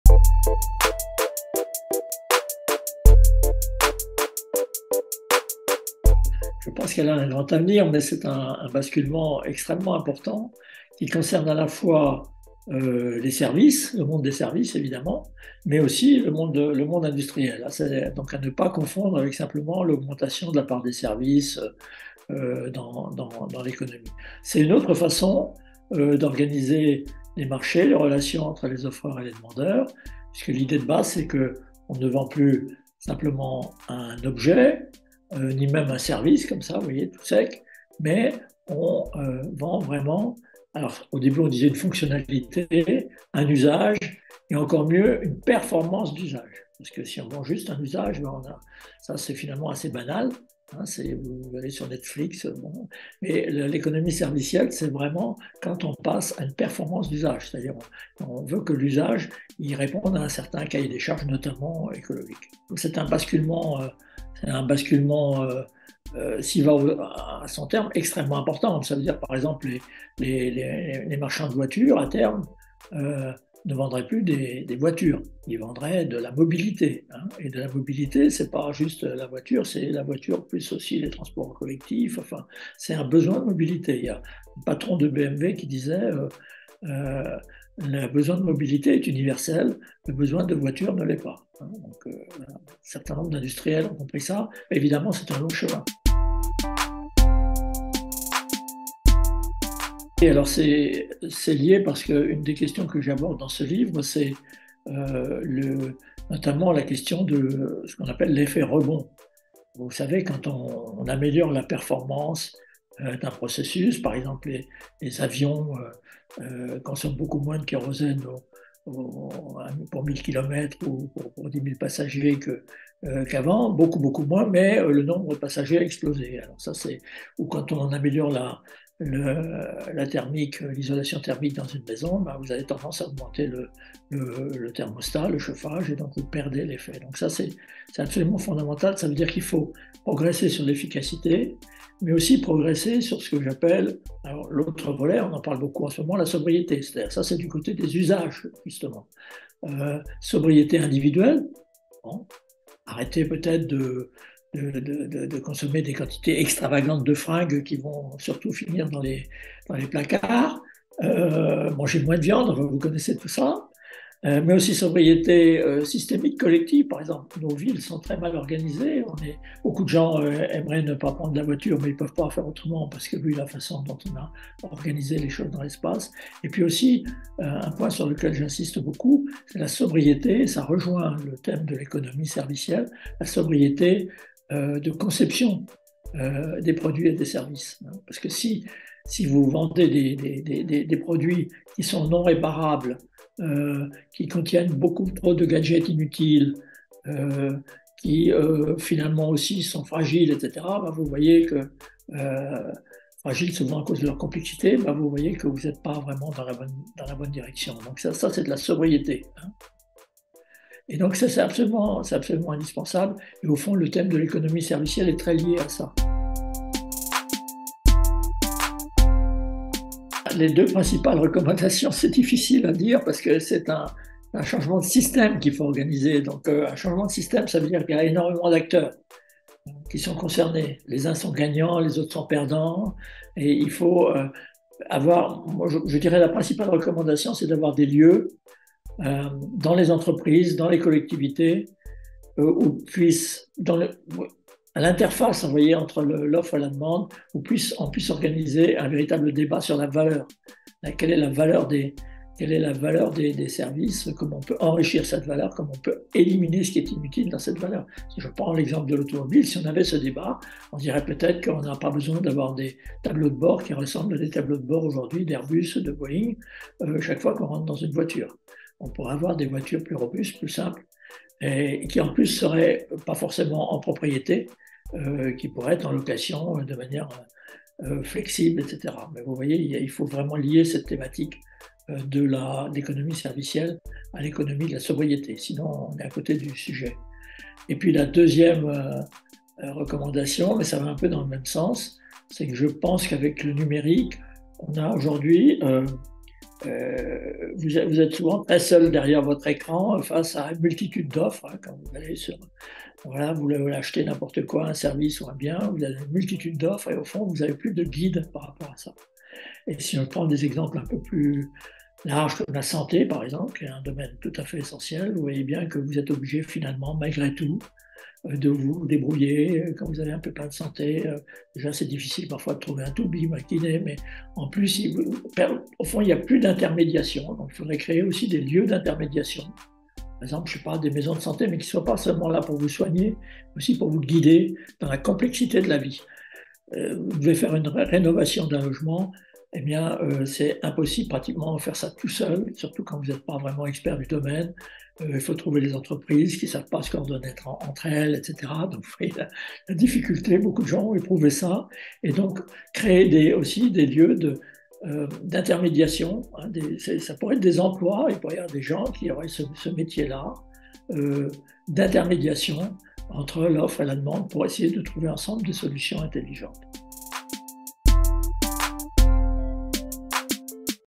Pierre Veltz (École des Ponts ParisTech) - Economie servicielle - cese
Questions à M. Pierre Veltz, Professeur émérite de l’École des Ponts ParisTech, auditionné par la commission Économie et finances du CESE dans le cadre de la saisine : "Économie servicielle".